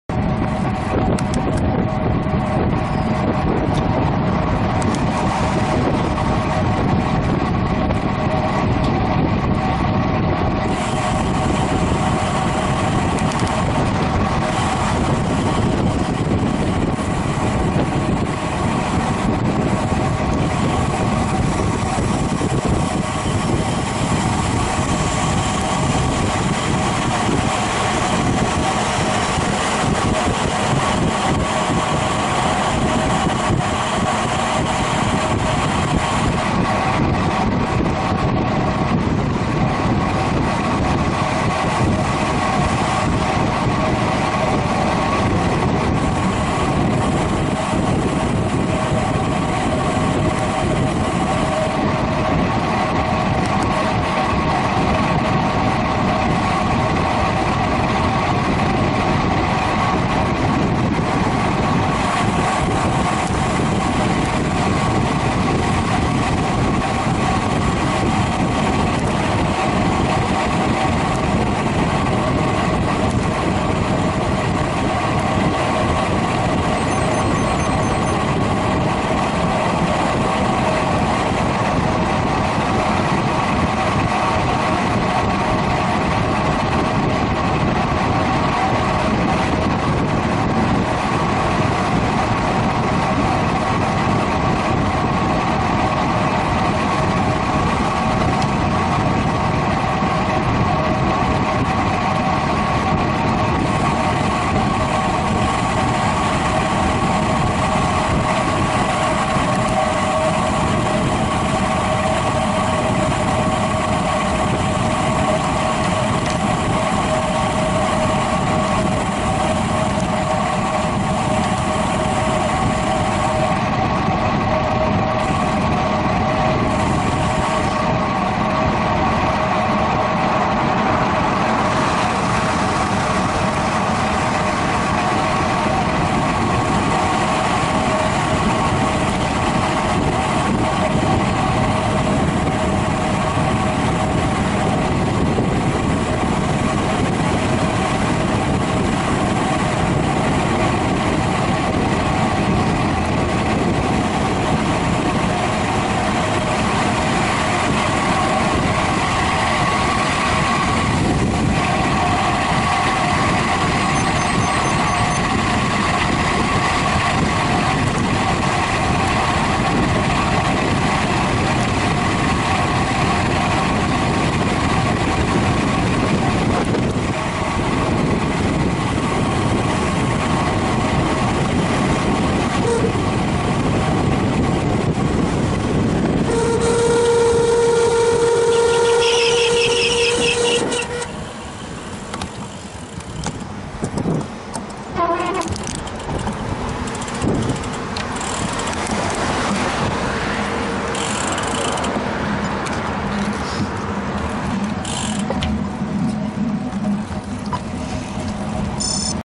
Descent into Gosford
A descent into Gosford while on an Audax ride earlier this year. You get the pleasure of hearing my squeaky rear brakes, that I was dealing with all day, at the end of the video.